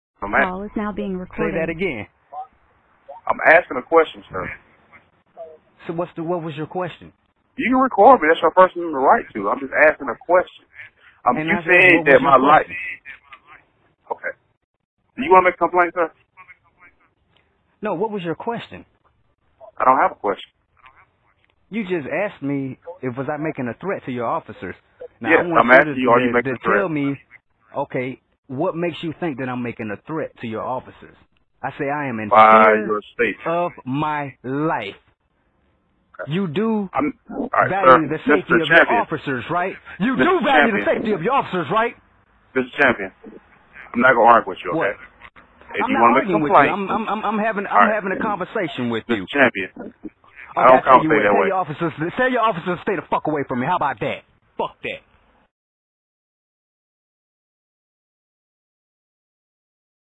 THIS is a LEGAL NOTICE for CLAYTON COUNTY POLICE DEPARTMENT § 9 11 65
§ 9-11-65 - Injunctions and restraining orders
this is not a threat. it is a legal notice made in fear by ROBERT DINO CHAMPION without prejudice. ANY violation will result in a arrest for aggravated stalking... This is no joke, it is in fact a legal notice § 9-11-65